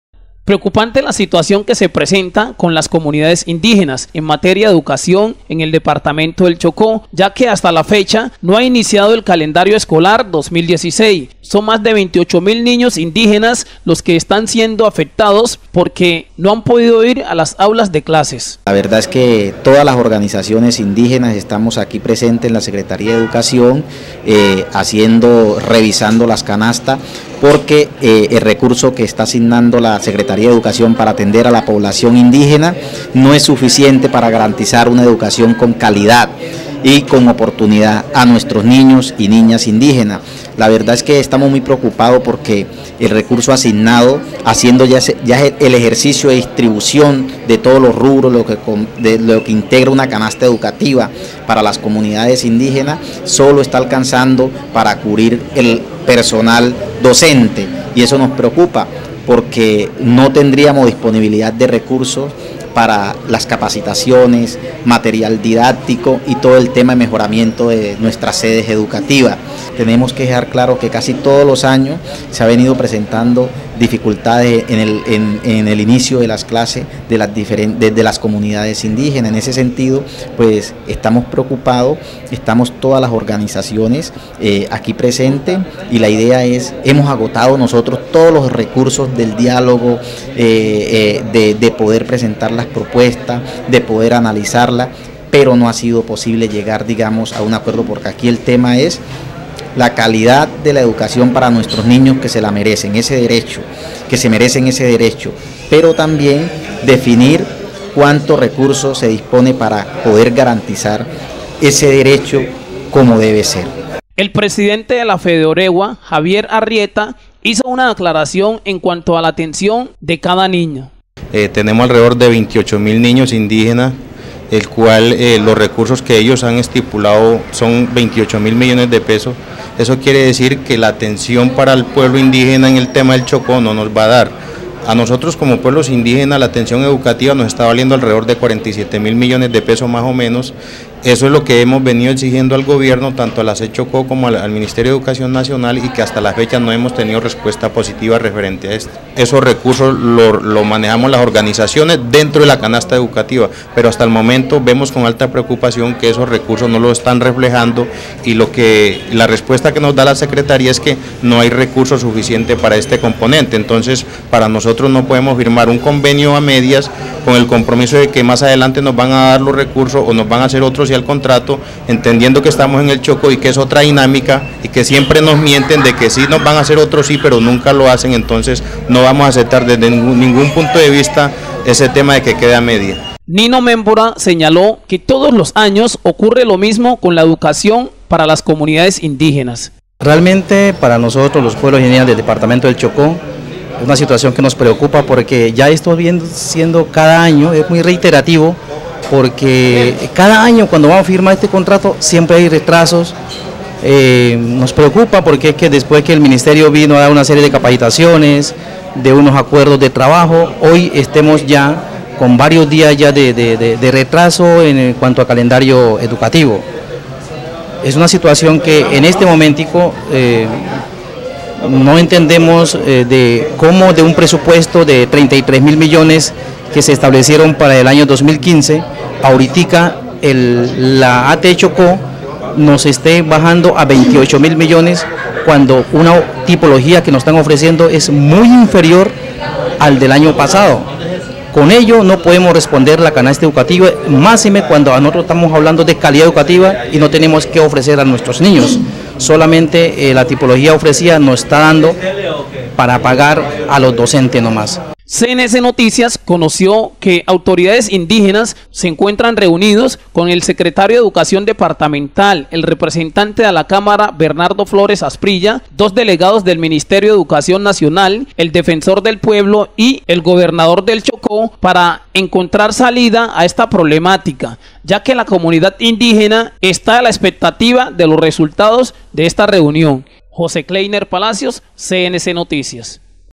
INDÍGENAS RECLAMAN FRENTE A LAS INSTALACIONES DE LA SED CHOCÓ, MEJORAS EN LA EDUCACIÓN
Preocupante la situación que se presentan  las comunidades indígenas en materia de educación en el departamento del choco, ya que hasta la fecha no ha iniciado el calendario escolar 2016  y  son más de  28 niños indígenas los que  se están viendo afectados porque  no han podido ir a  las aulas de clases
El presidente de FEDEOREWA Javier Arrieta hizo una aclaración en cuanto a la atención de cada niño indígena
Niño Membora señalo que todos los años ocurre lo mismo con la educación para las comunidades indígenas
Cnc noticias conoció que autoridades indígenas se encuentran reunidos con el secretario de educación departamental, el representante a la cámara Bernardo Flórez Asprilla, dos delegados del ministerio de educación nacional, el defensor del pueblo y el gobernador del choco.  Para encontrar salida a este problema ,  ya que la comunidad indígena esta a la expectativa de los resultados de esta reunión y esperamos la respuesta del secretario de educación departamental